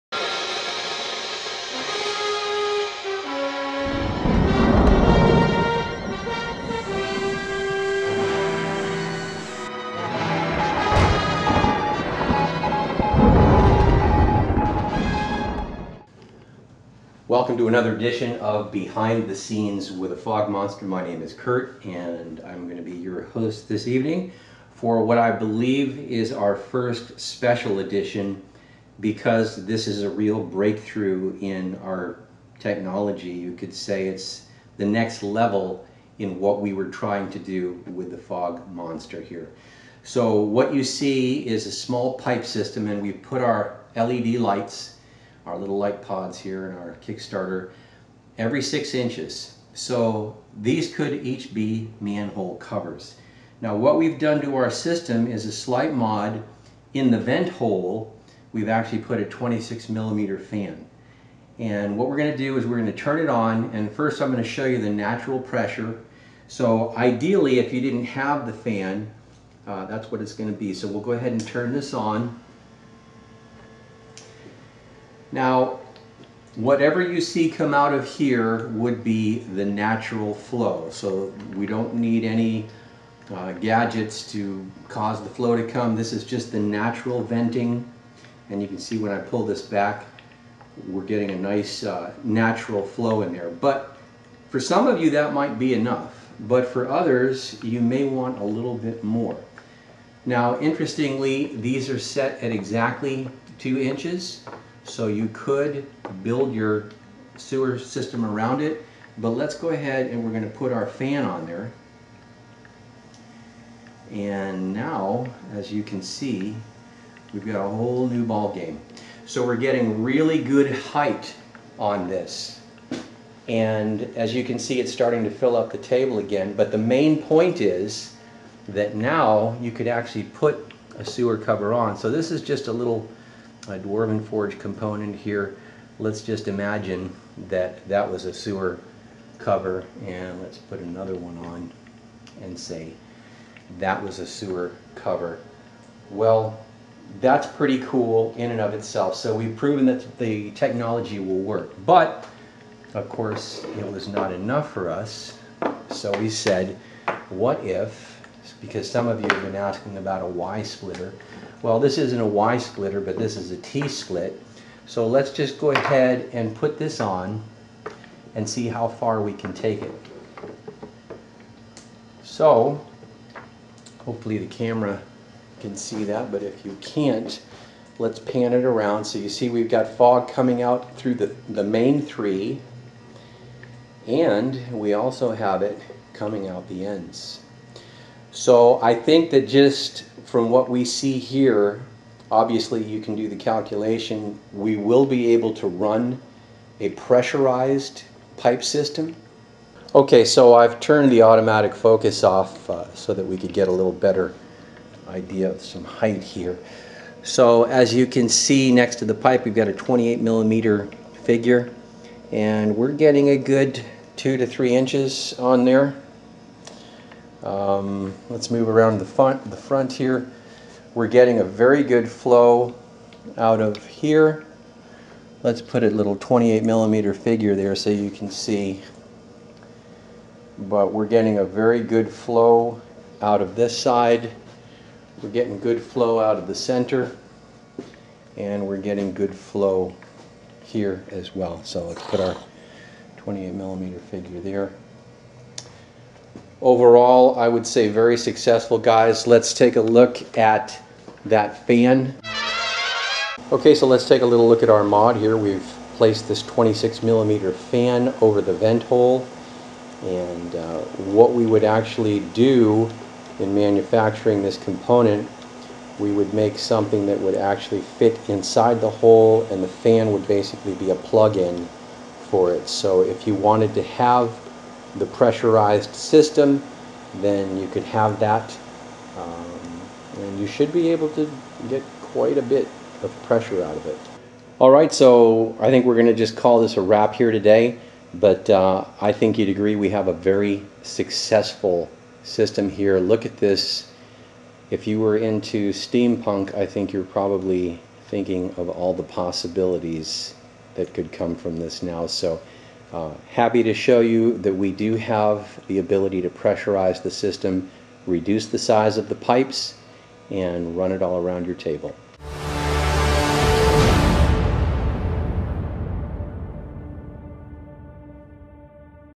Prototype Fog Monster Fog Machine For Tabletop Games Demo Using Smaller Diameter Pipe & Fan.
Now available Special Report: You asked for it! We are very excited to bring you a major breakthrough for the Fog Monster on several fronts. Many of you have asked if it was possible to have 1. smaller pipes, 2. A pipe system with a spiltter that could be laid around the table and have fog bellowing out from multiple outlets for example a sewer system that had manhole covers placed at 6" intervals with what appeared to be steam rising up through the covers.

Watch the video to see and let us know if you would like this.